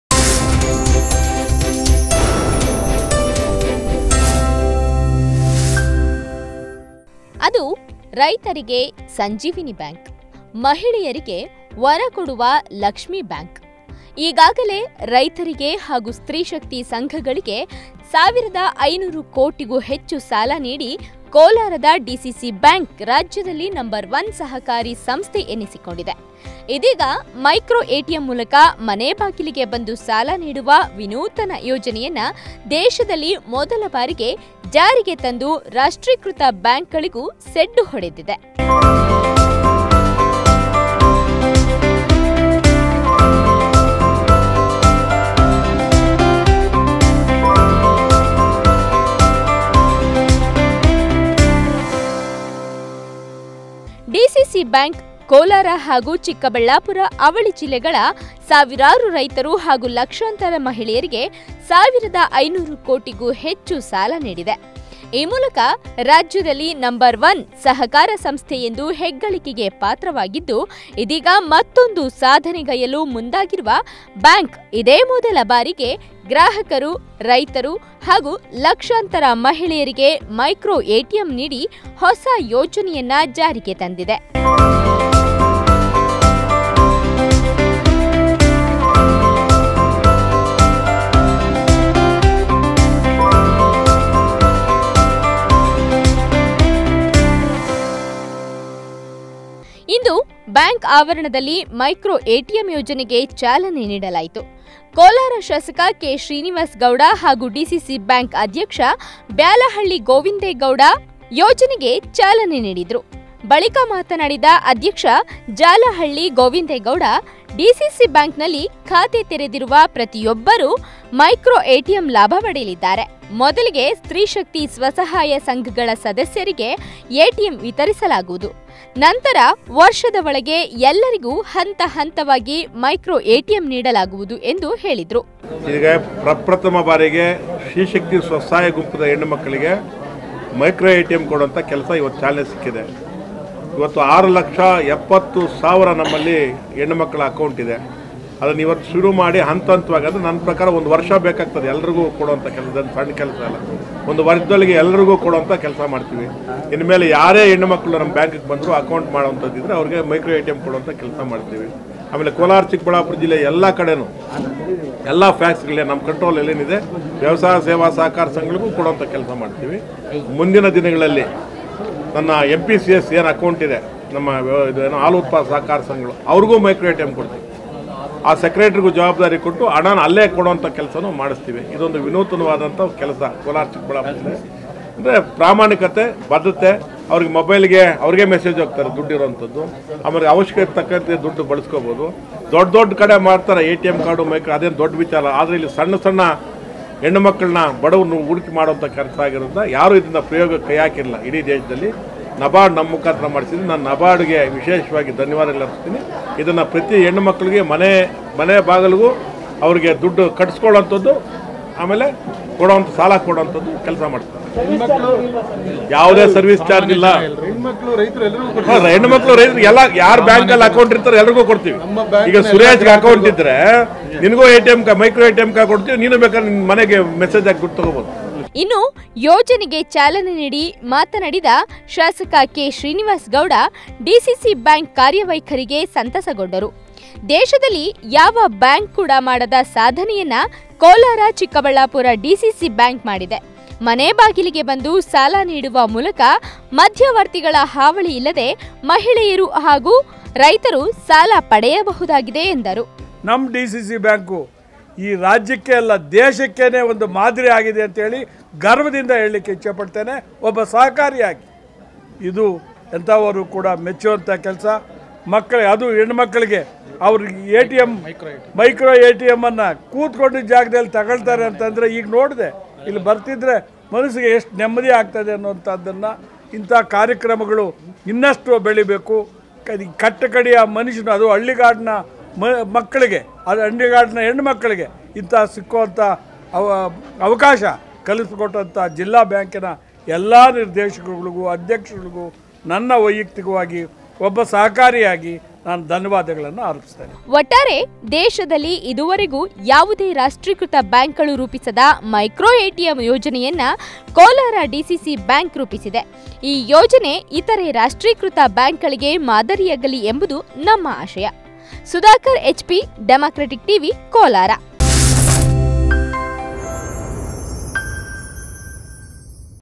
ಕೋಲಾರದಲ್ಲಿ ಮೈಕ್ರೋ ಎಟಿಎಂ / Micro ATM in Kolar
ಅದು ರೈತರಿಗೆ ಸಂಜೀವಿನಿ ಬ್ಯಾಂಕ್. ಮಹಿಳೆಯರಿಗೆ ವರ ಕೊಡುವ ಲಕ್ಷ್ಮೀ ಬ್ಯಾಂಕ್. ಈಗಾಗಲೇ ರೈತರಿಗೆ ಹಾಗೂ ಸ್ತ್ರೀ ಶಕ್ತಿ ಸಂಘಗಳಿಗೆ 1500 ಕೋಟಿಗೂ ಹೆಚ್ಚು ಸಾಲ ನೀಡಿ ಕೋಲಾರದ ಡಿಸಿಸಿ ಬ್ಯಾಂಕ್ ರಾಜ್ಯದಲ್ಲಿ ನಂ 1 ಸಹಕಾರ ಸಂಸ್ಥೆ ಎನ್ನಿಸಿಕೊಂಡಿದೆ. ಇದೀಗ  ಮೈಕ್ರೋ ಎಟಿಎಂ ಮೂಲಕ ಮನೆ ಬಾಗಿಲಿಗೆ ಬಂದು ಸಾಲ ನೀಡುವ ವಿನೂತನ ಯೋಜನೆಯನ್ನು ದೇಶದಲ್ಲಿ ಮೊದಲ ಬಾರಿಗೆ ಜಾರಿಗೆ ತಂದು ರಾಷ್ಟ್ರೀಕೃತ ಬ್ಯಾಂಕ್ಗಳಿಗೂ ಸೆಡ್ಡು ಹೊಡೆದಿದೆ.